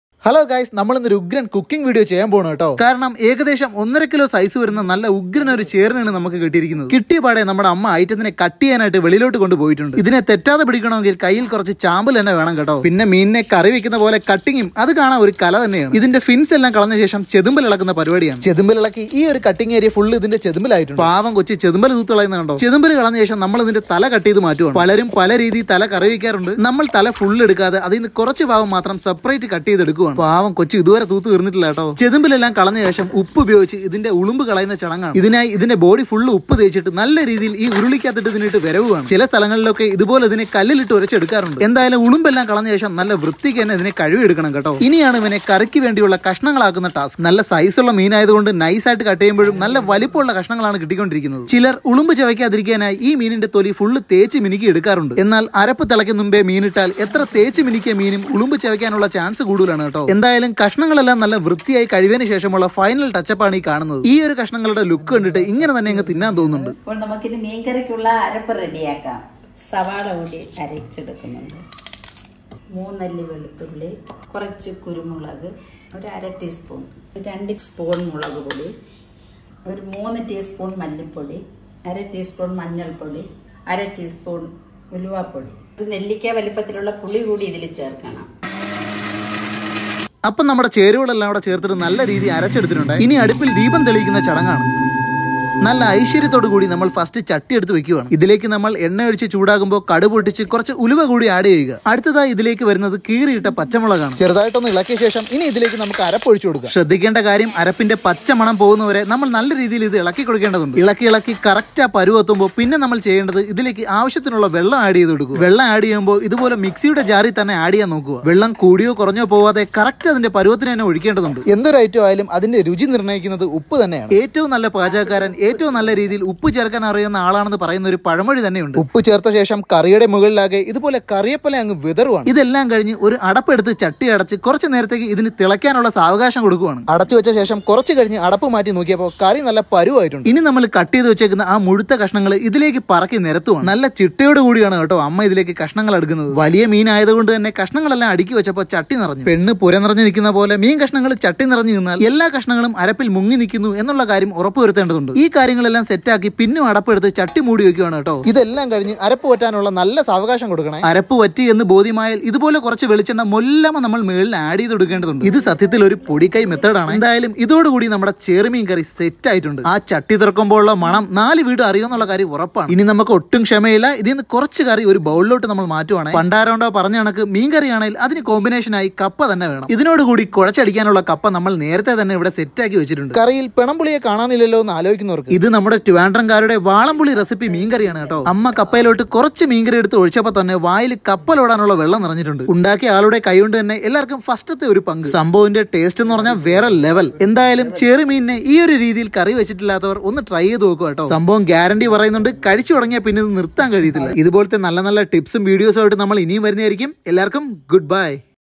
ഇങ്ങനെ ആകും എന്ന് പ്രതീക്ഷിച്ചില്ല | Kerala Style Snakehead Curry
The video presents the Awesome Recipe of Kerala Style Snakehead Fish Curry.
Catch a Fish and Cook